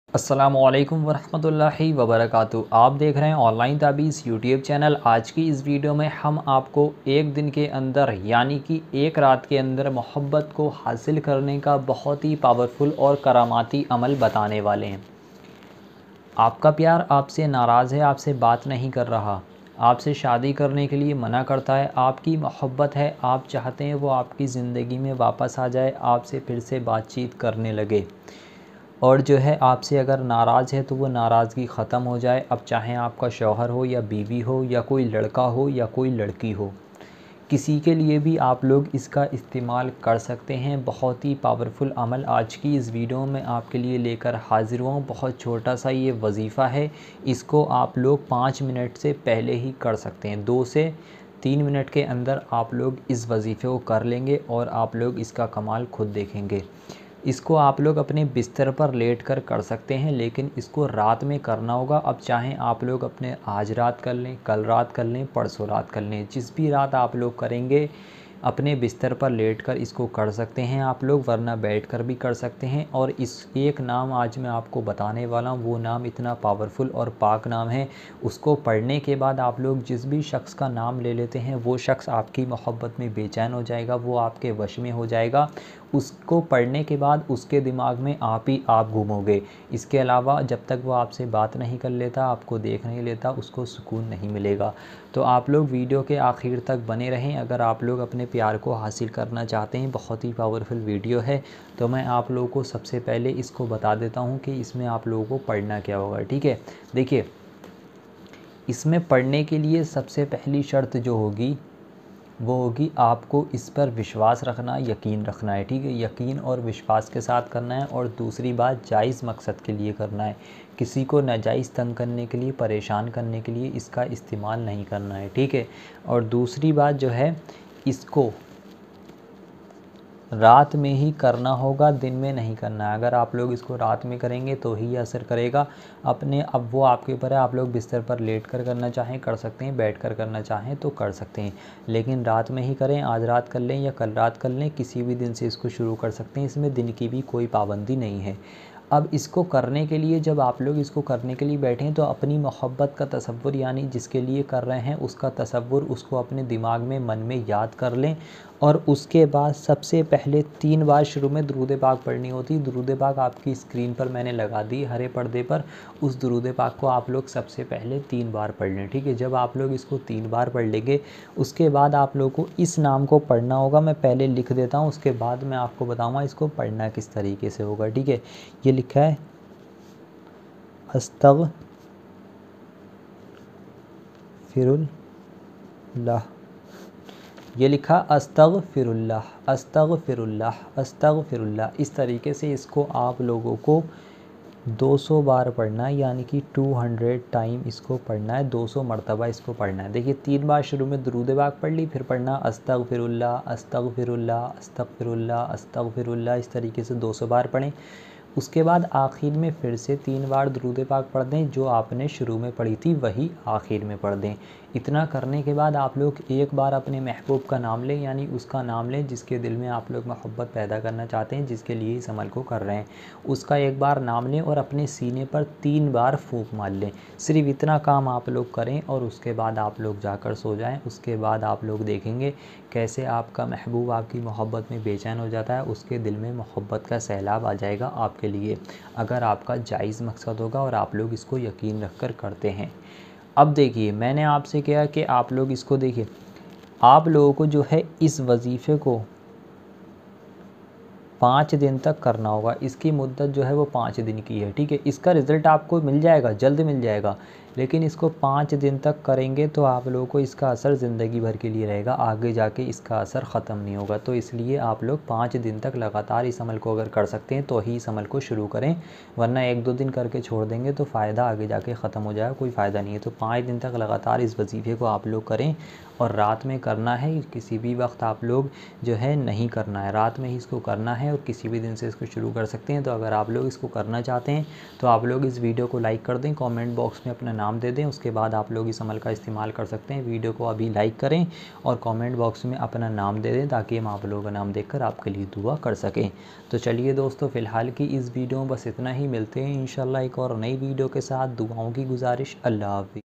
सिर्फ 1 नाम बोल दो आपका प्यार आपको देखने आपसे बात करने के लिए तरसने लगेगा | 1 घंटे में असर शुरू होगा
सिर्फ 1 नाम बोल दो आपका प्यार आपको देखने आपसे बात करने के लिए तरसने लगेगा | 1 घंटे में असर शुरू होगा - Online Taweez

📵NOTE :- Comment में कोई भी नंबर देता है तो आप उस नंबर पर Contact न करें। ये लोग Fraud है । आपका काम भी नही करेंगे और पैसा भी ले लेंगे। तो होशियार रहे.